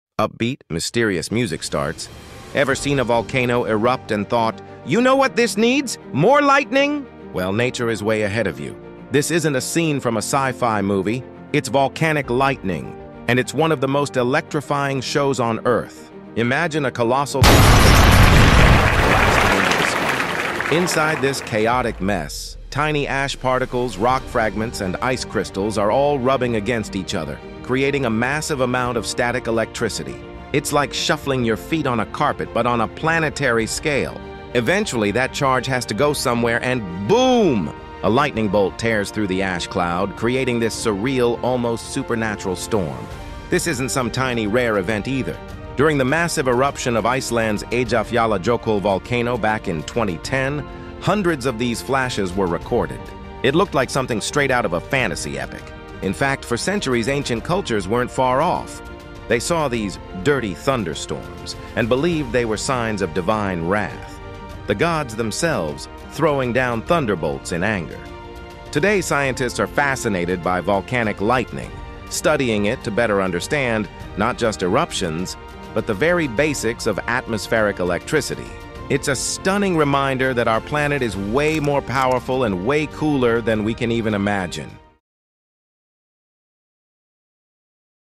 Volcanic Lightning (Global Volcanic Zones)
A rare and electrifying spectacle where lightning bolts erupt from volcanic ash clouds during eruptions, creating a surreal fusion of fire and electricity.